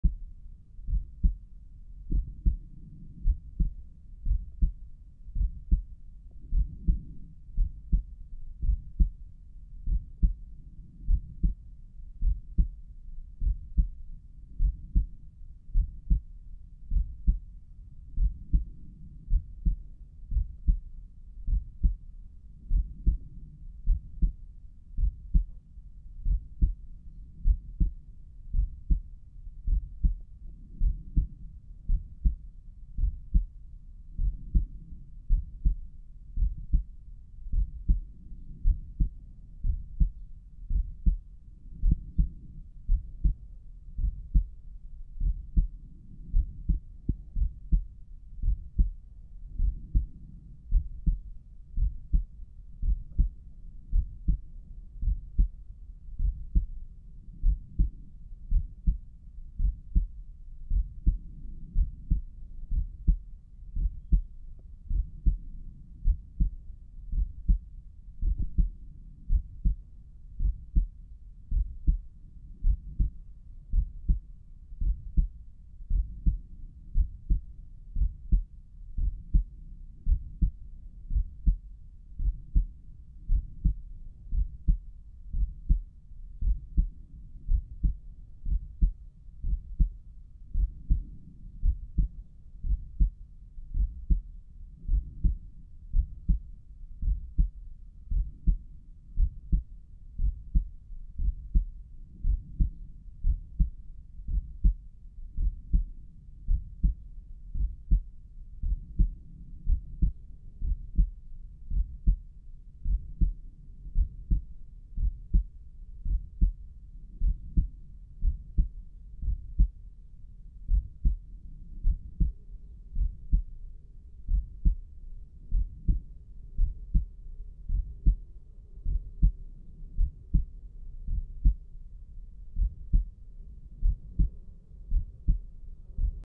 Visible carotid pulse and heartbeat
My heart was pounding hard today, and I could feel my carotid arteries surging with each heartbeat. Sure enough, they were visibly pulsing too (less so than before I got set up to record, but that's always the way!). Watch them throb as you listen to my heartbeat at Erb's point. How's my heart doing?